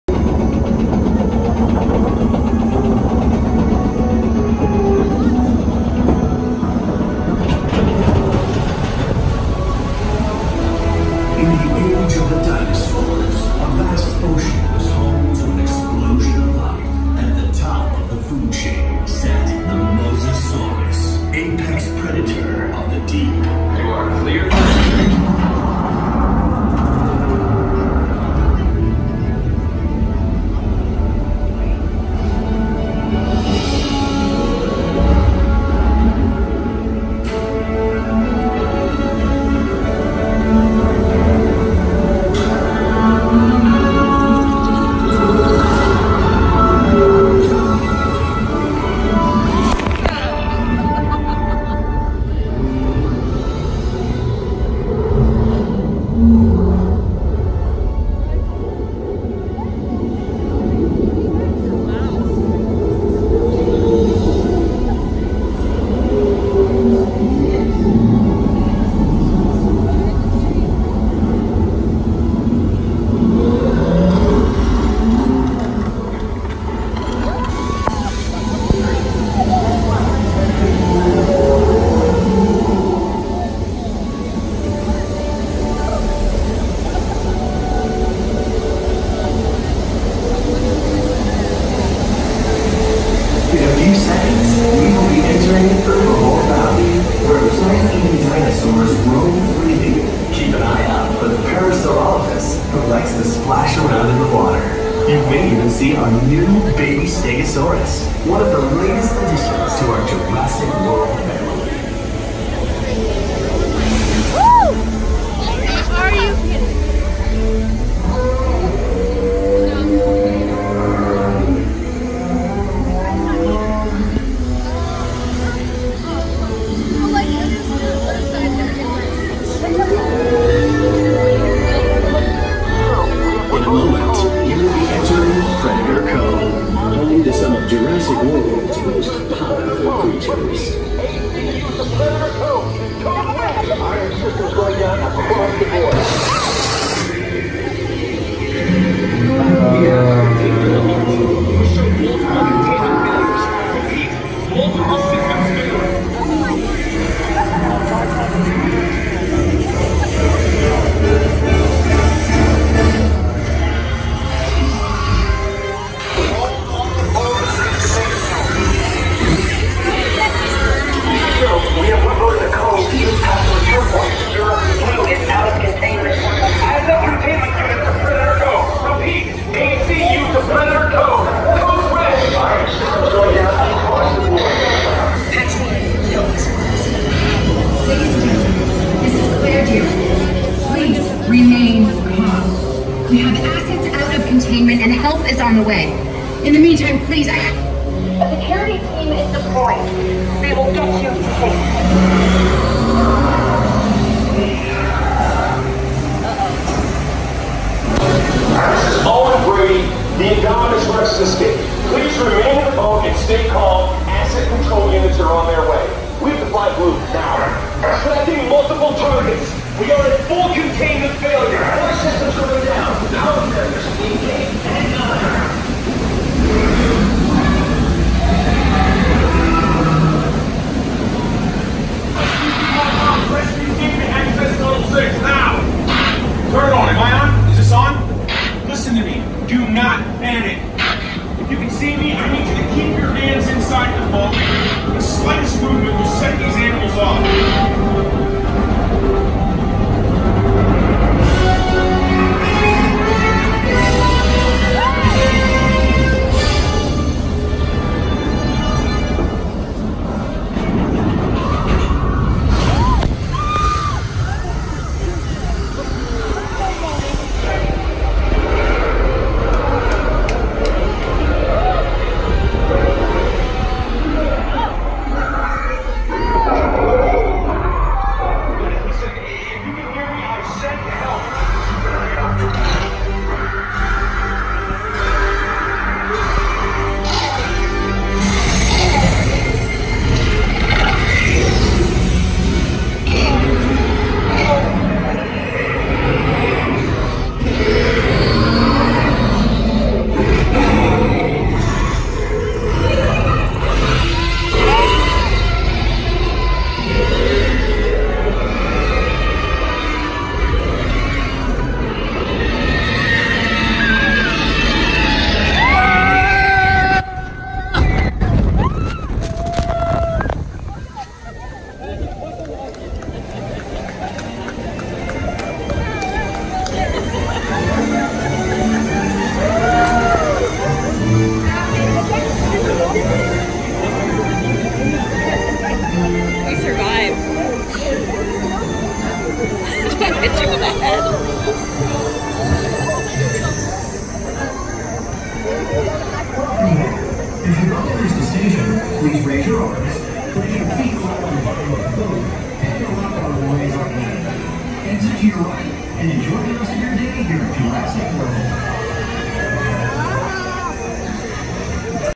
Jurassic World Front Seat Entire Ride Universal Studios
Entire Ride @ Universal Studios Theme Park, from start to finish.